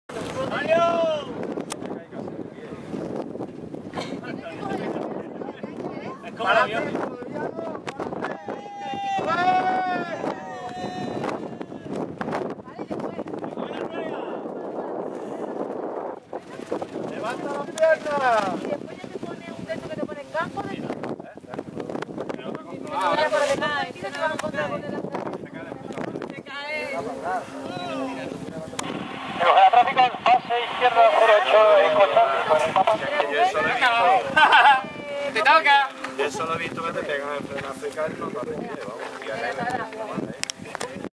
PARACAIDISMO EN TREBUJENA
LA EMPRESA SKYDIVE JEREZ UTILIZA EL AERODROMO DE TREBUJENA PARA SUS SALTOS